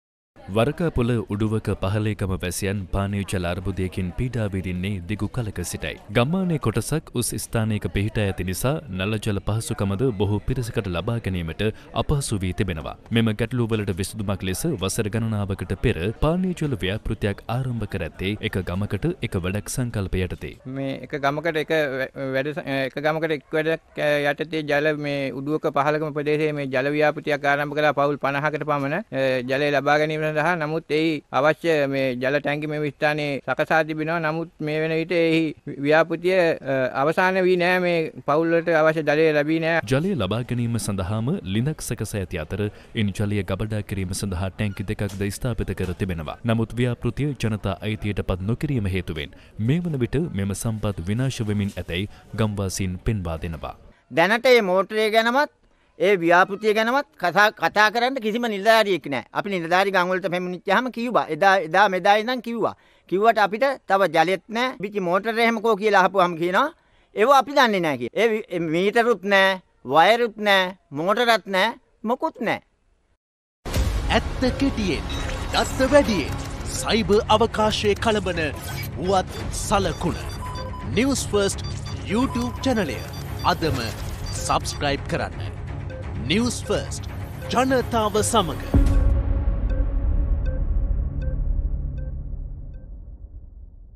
වසර ගණනාවක සිට පානීය ජල අර්බුදයකින් පීඩා විඳින ගම්මුන්
රුපියල් ලක්ෂ 10ක් වැය කර පවුල් 50ක ජල පරිභෝජනය වෙනුවෙන් ඉදිකළ පානීය ජල ව්‍යාපෘතියක් විනාශ වෙමින් පවතින බව වරකාපොල උඩුවක පහළ ගම ගම්වාසීන් පවසනවා.